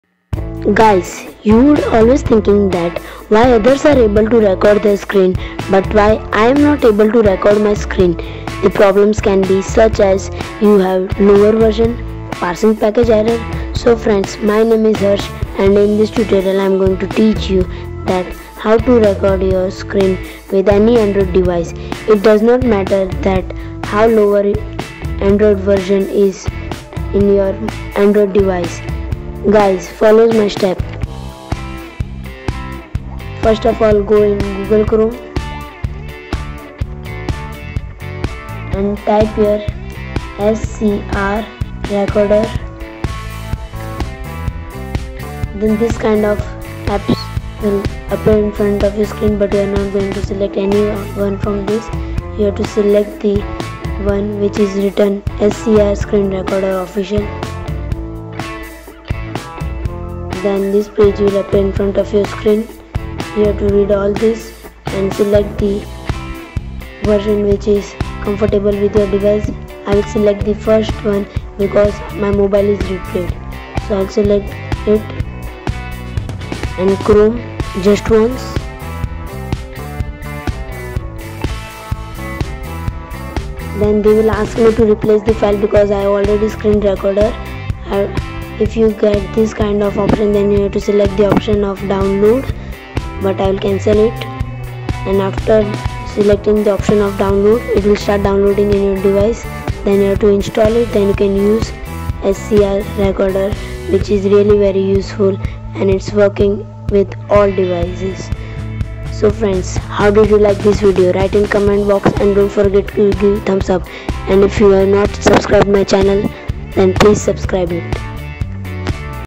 Screen Recorder For KITKAT Version |YOU CAN ALSO
Hello Friends

Today we will Learn How to Download a Screen Recorder For Andriod

Requirements

* Your Android Version should be ( Lollipop)
*If Your Android Version is (Kitkat or Lower) "YOUR DEVICE SHOULD BE ROOT"

1. Go to the browser
2.Type " scr screen recorder "
3. Select the first option
4. The page will load.
  A. your Device has Root then select the First one
  b.if your Android version is lollipop then download 2nd one
5. Then click download ⬇
6. Instal and enjoy!

If you want to go directly to the link🔗